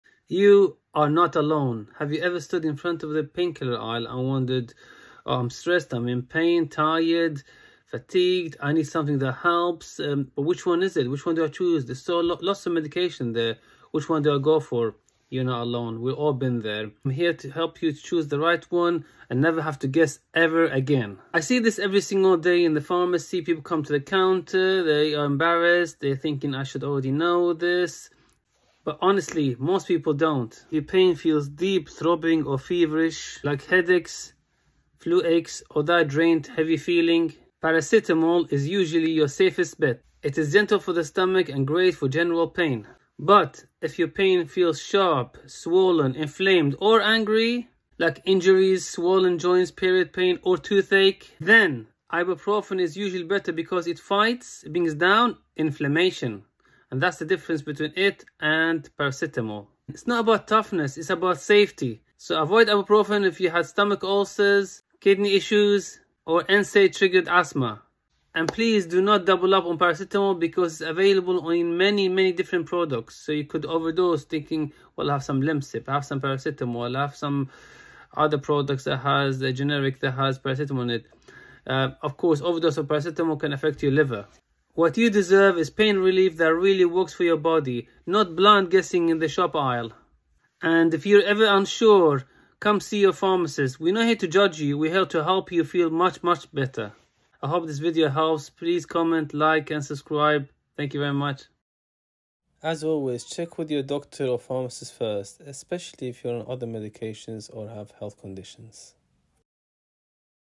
Paracetamol vs Ibuprofen: When to Take Each (Pharmacist Explains)
💊 Confused about whether to take paracetamol or ibuprofen? In this short, I explain the key differences between the two, their uses, and safe ways to decide which to take.

As a qualified pharmacist, I share practical advice so you can manage mild pain or fever safely and effectively.

✅ Remember: Always follow the instructions on the pack and consult a healthcare professional if symptoms persist.